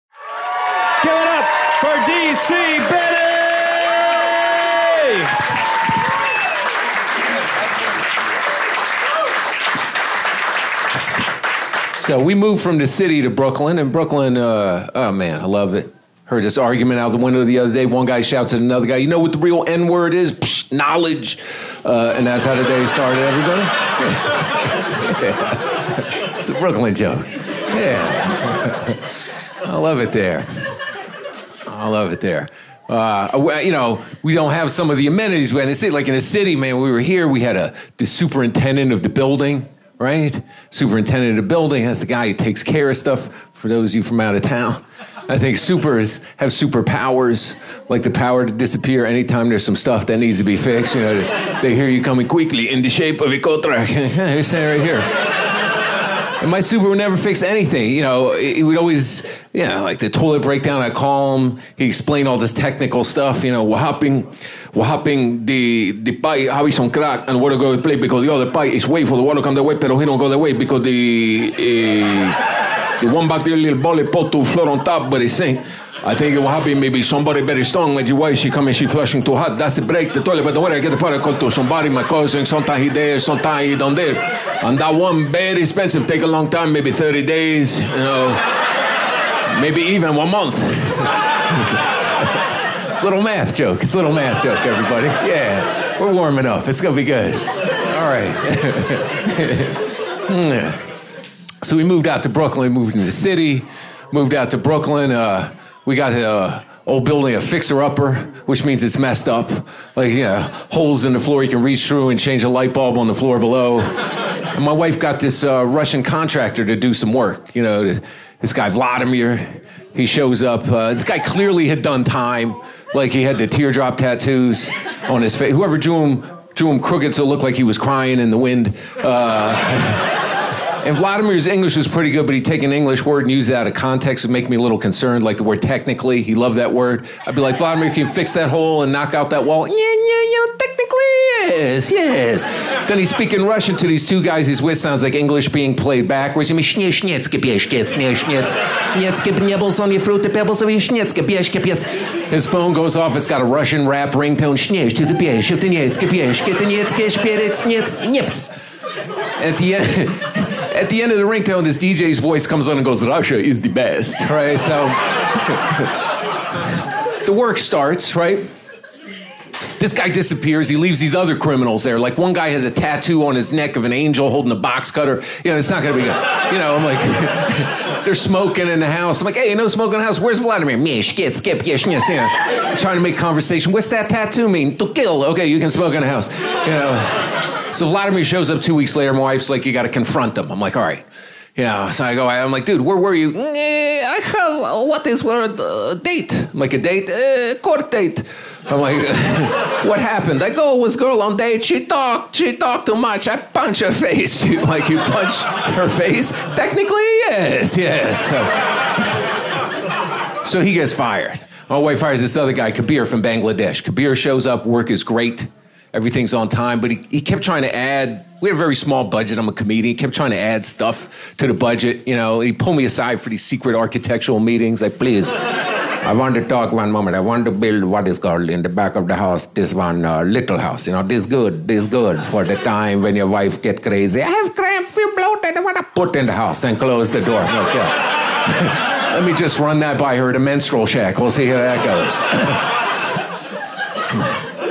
The Russian Contractor Story | Live at The Fat Black | D.C. Benny
D.C. Benny’s unusual style of telling autobiographical stories, inhabited by original characters has been sharpened for almost thirty years as a performer.  Listen to some of his best stories